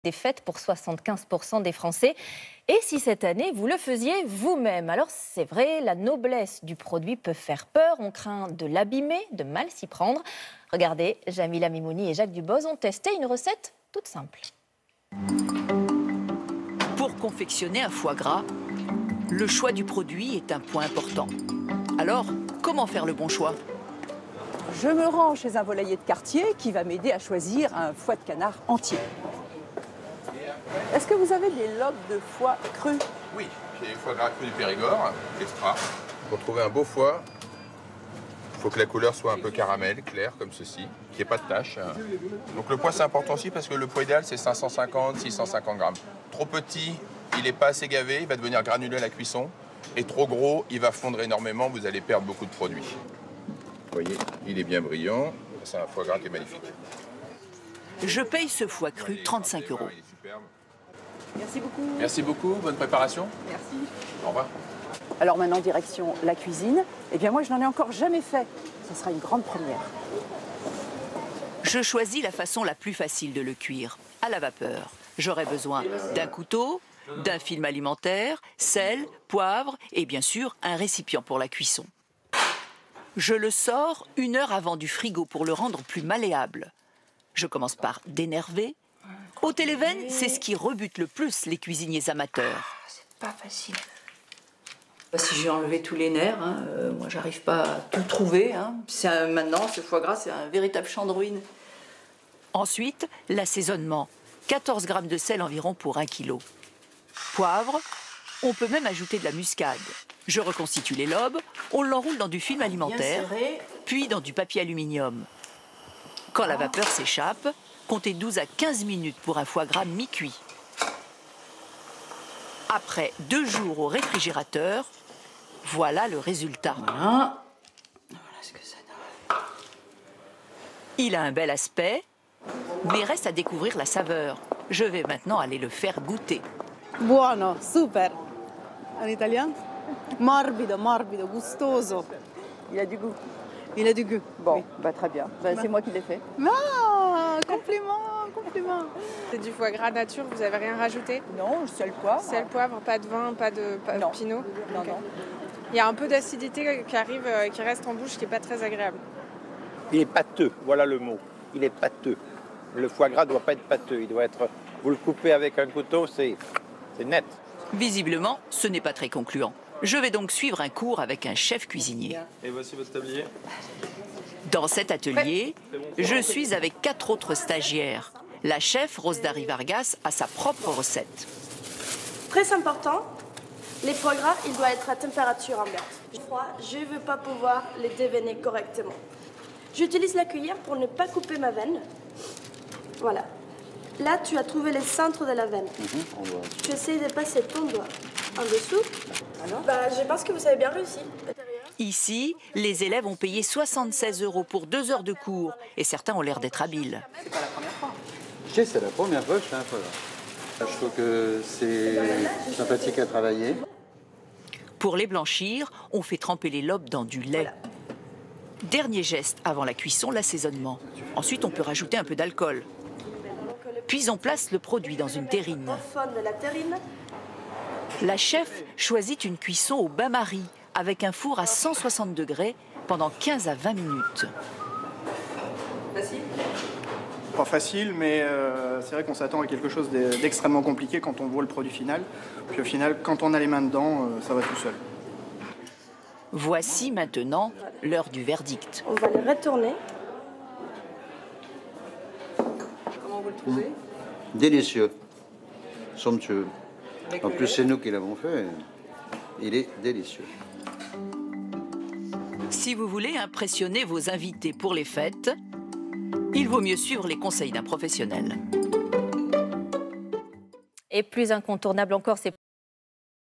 Faire son foie gras, déc 2018
Reportage France 2 sur le foie gras fait maison.